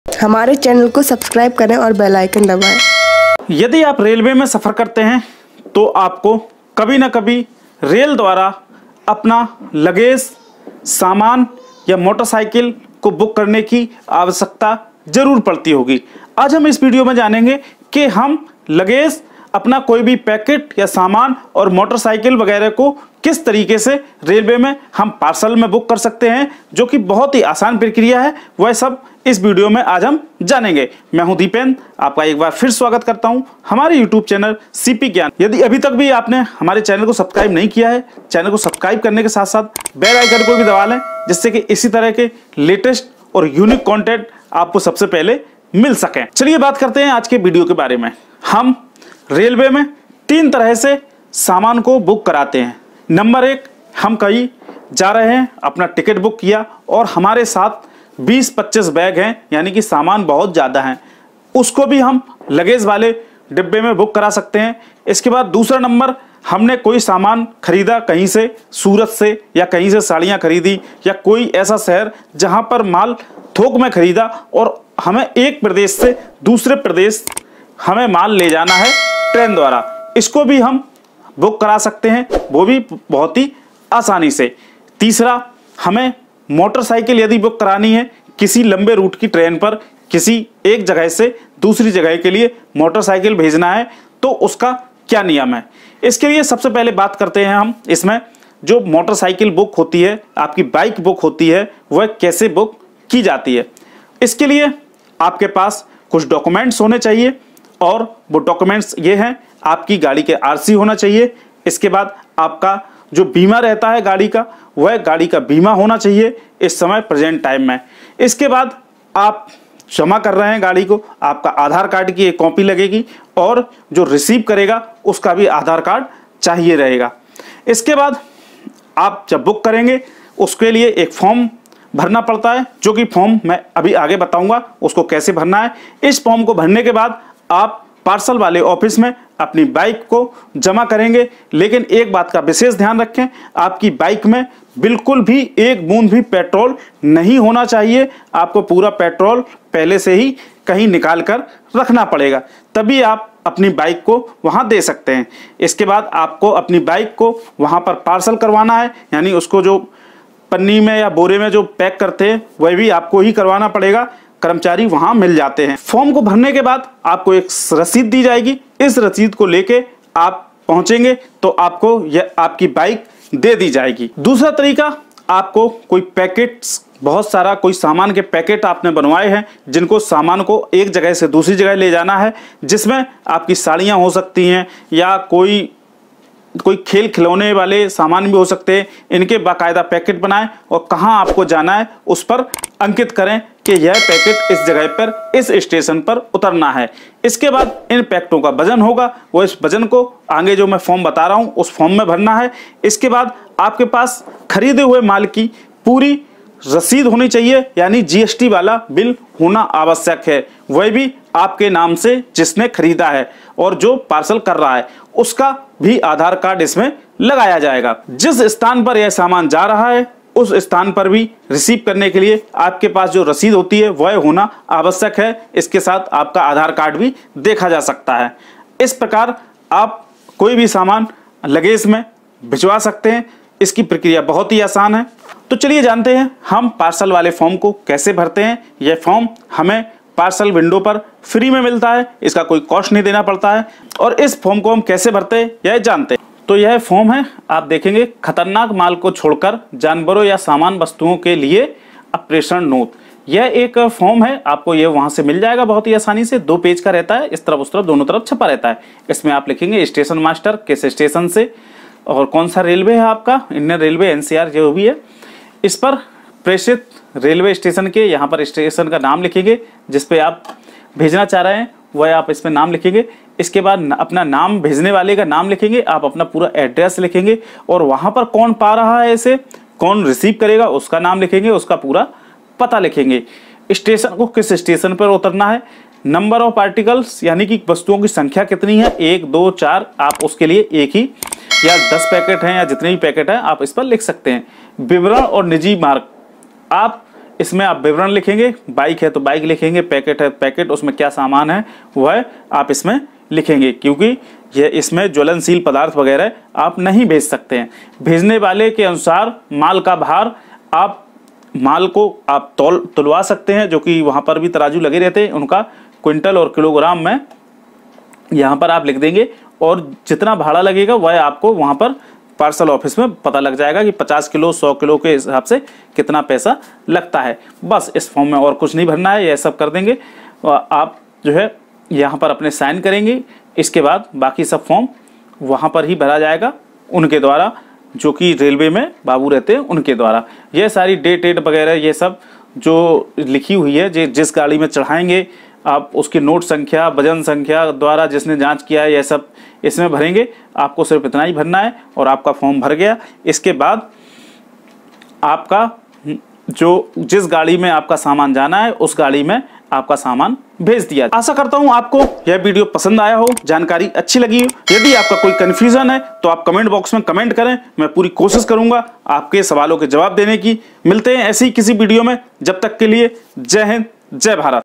Train Mein Parcel Kaise Book Karen? Railway Luggage Parcel Rules | Railway parcel booking online
I am Deependra. Welcome to our YouTube Chanel  @cpgyan